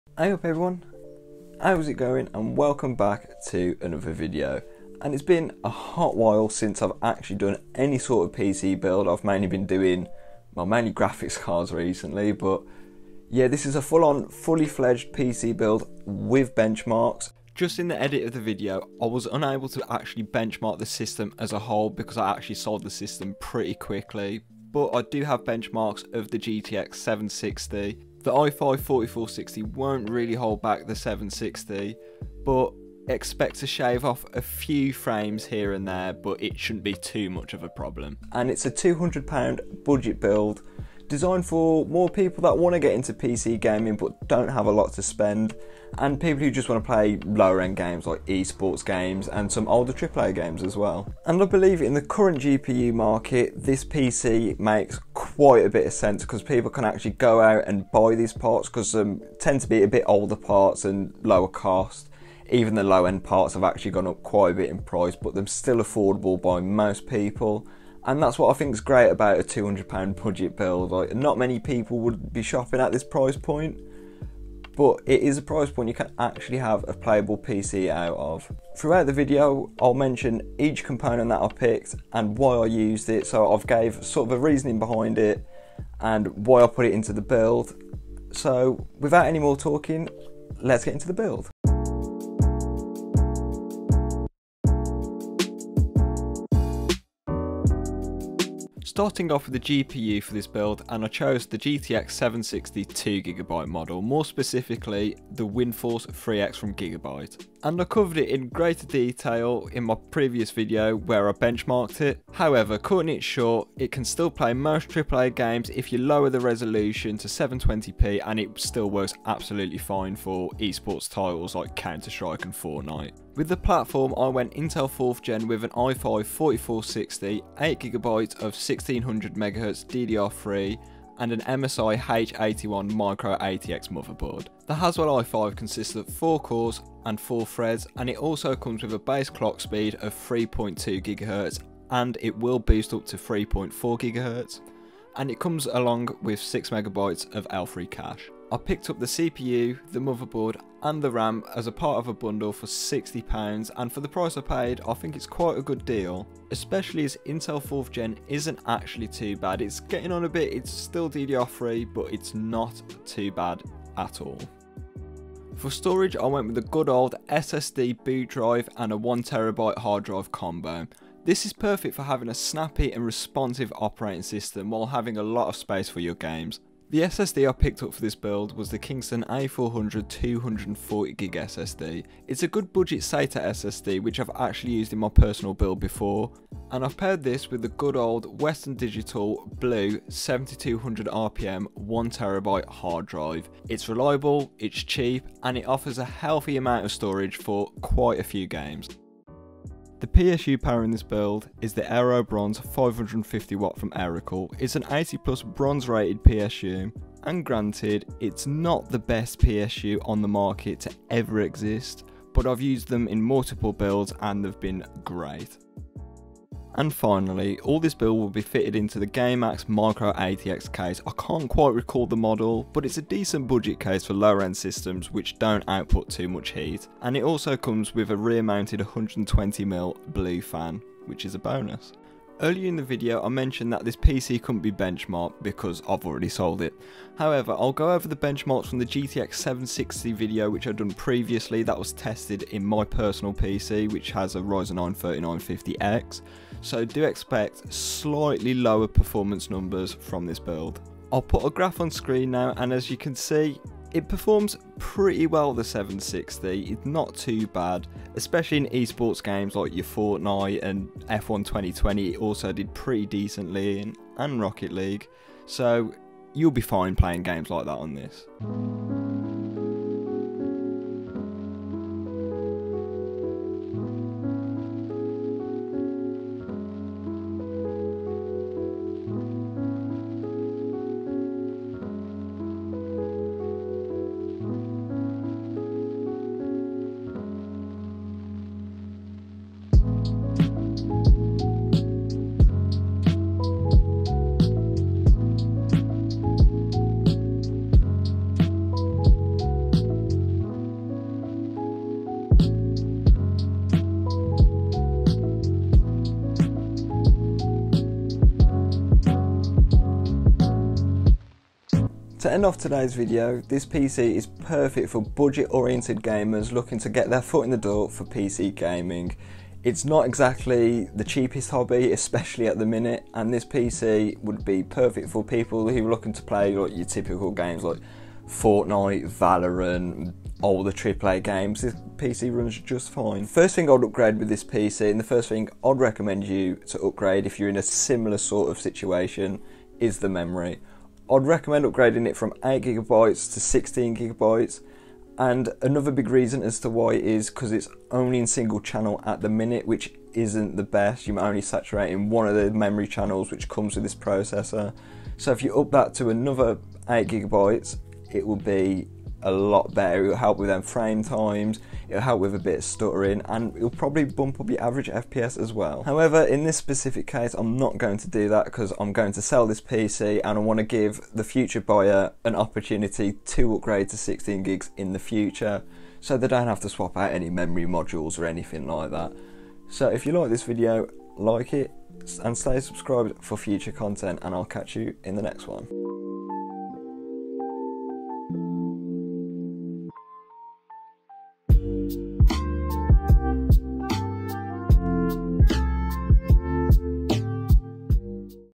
The £200 Budget Gaming PC Build
As the PC market shows no signs of recovering anytime soon, a budget PC build might be a great alternative until prices normalize. This PC features an i5 4460, 8GB of RAM and a GTX 760 2GB for £200. In this market, that isn't the worst deal out there!